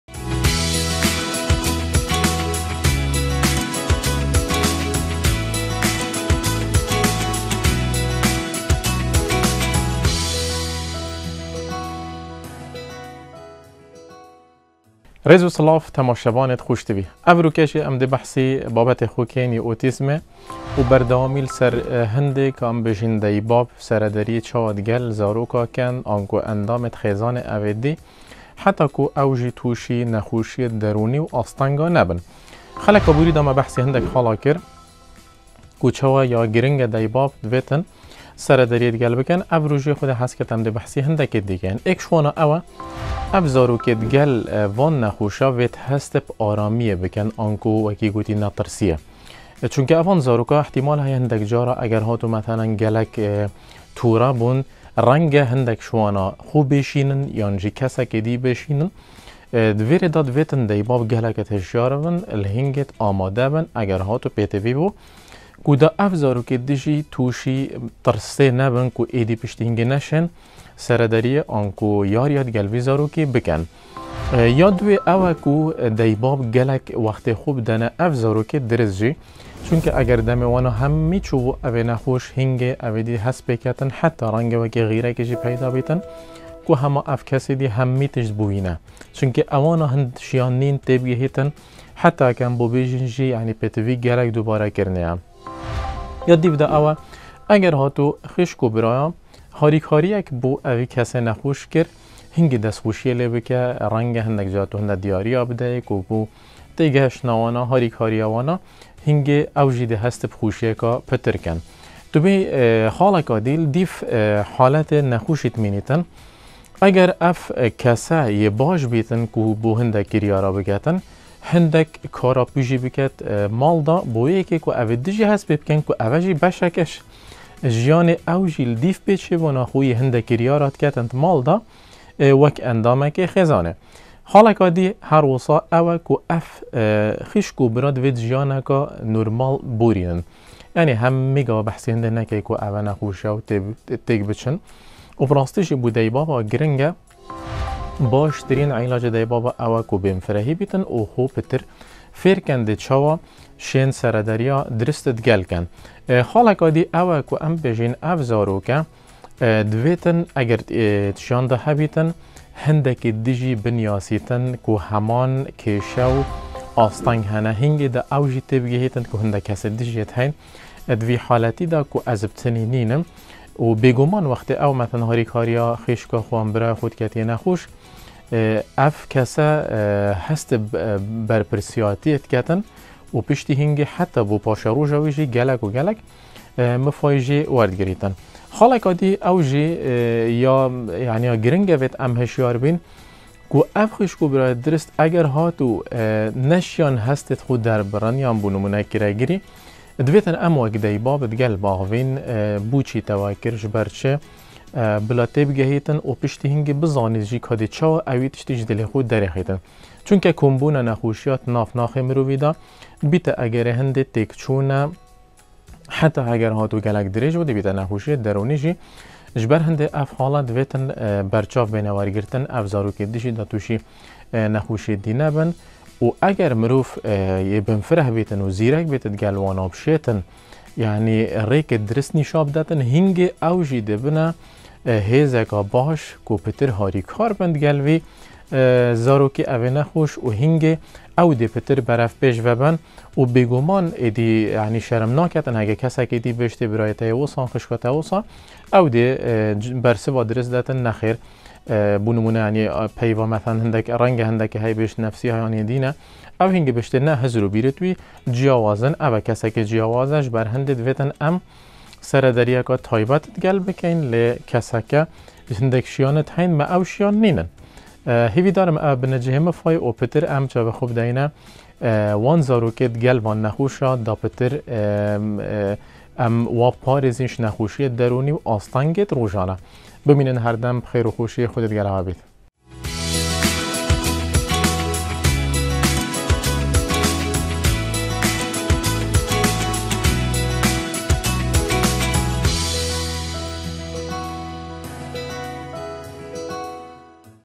بەرنامێ كلینكا خابیر | دکتور ئیسلام یاسین | سەرەدەریا درست دگەل خیشک و برایێت نەخوشێ ئوتیزمێ - 2
بەرنامێ كلینكا خابیر
بەرهەڤکرن و پێشکێشکرنا: دکتور ئیسلام یاسین
 بابەت: سەرەدەریا درست دگەل خیشک و برایێت نەخوشێ ئوتیزمێ - 2
ـــــ
هەمى ئەکوانتێن دەزگەهێ خابیر